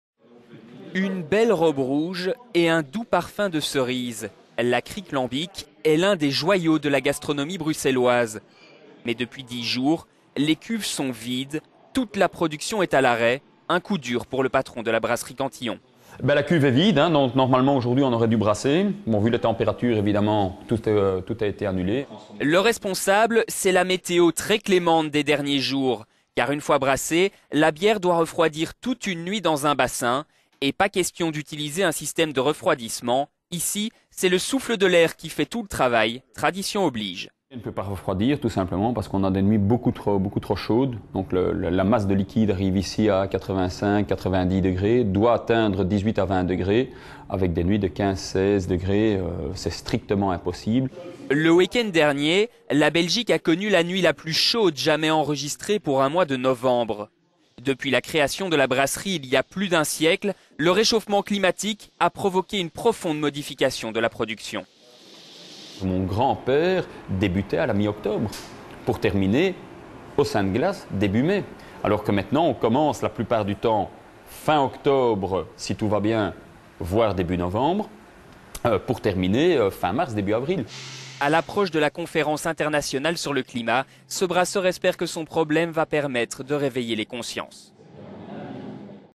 Réchauffement climatique: une brasserie belge à l'arrêt
Une belle robe rouge et un doux parfum de cerise. La kriek lambic est l'un des joyaux de la gatronomie bruxelloise. Brassée selon un procédé unique et artisanal. Mais depuis 10 jours les cuves sont vides, toute la production est à l'arrêt. Un coup dur pour le patron de la Brasserie Cantillon.

"La cuve est vide", déplore Jean Van Roy, patron de la brasserie Cantillon. "Normalement, aujourd'hui, on aurait dû brasser. Mais vu les températures, tout a été annulé."